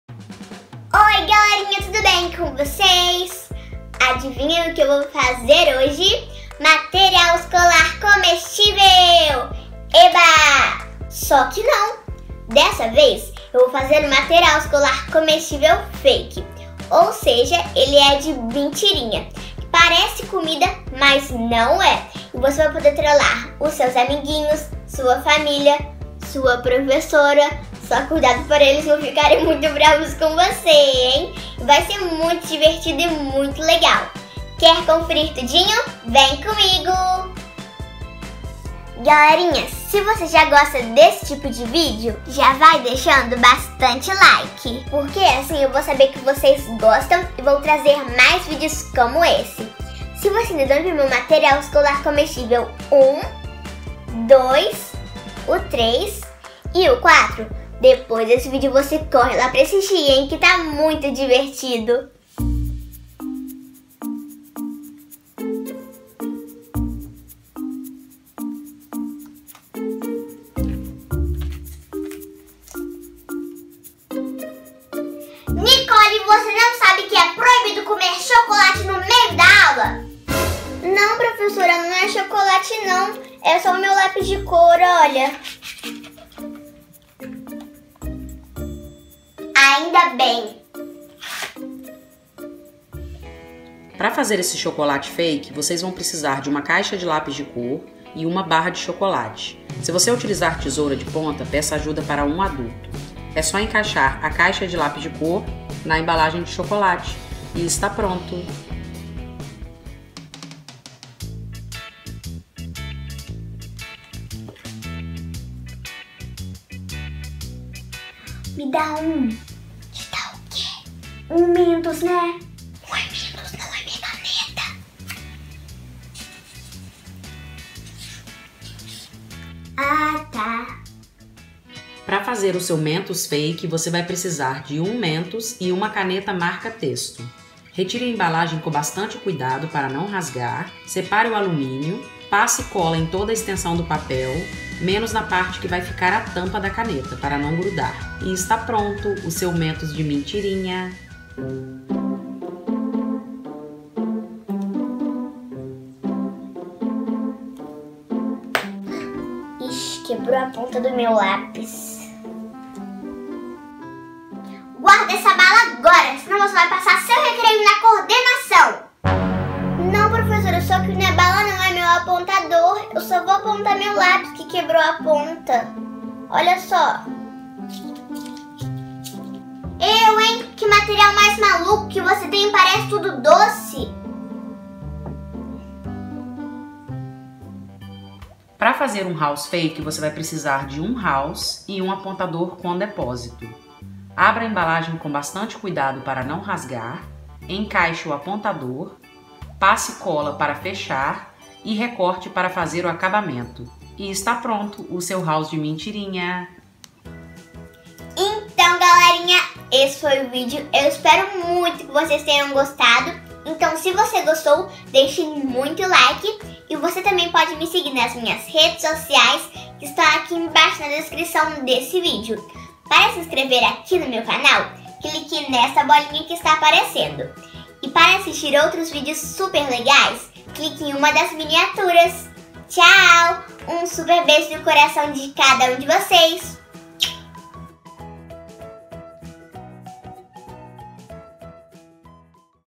DIY MATERIAL ESCOLAR COMESTÍVEL FAKE 2017( parece comida) DIY EDIBLE SCHOOL SUPPLIES
DIY MATERIAL ESCOLAR COMESTÍVEL FAKE 2017 ( parece comida) DIY EDIBLE SCHOOL SUPPLIES: Nesse vídeo, vou ensinar a fazer material escolar que parecem comida mas não é.